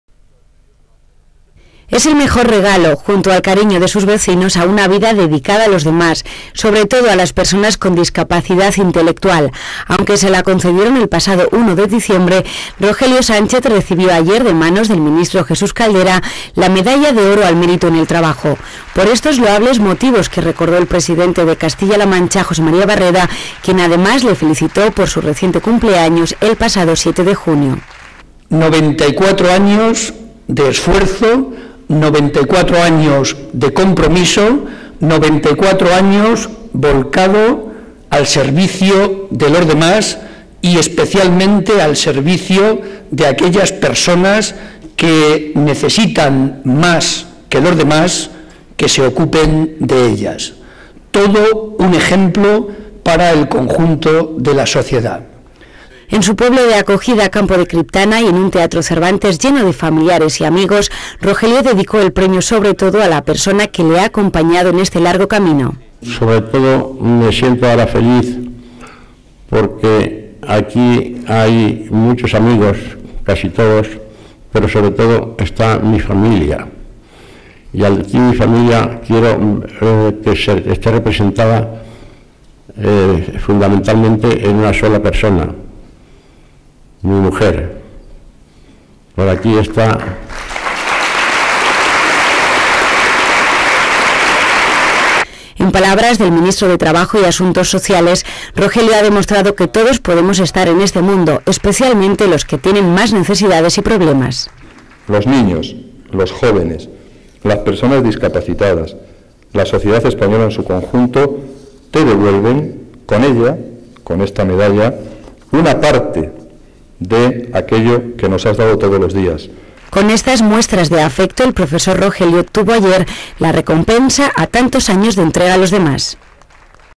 Barreda y Caldera entregan Medalla Oro Mérito en Trabajo
El presidente de Castilla-La Mancha, José María Barreda y el Ministro de Trabajo, Jesús Caldera, hacen entrega de la Medalla de Oro al Mérito en el Trabajo a Rogelio Sánchez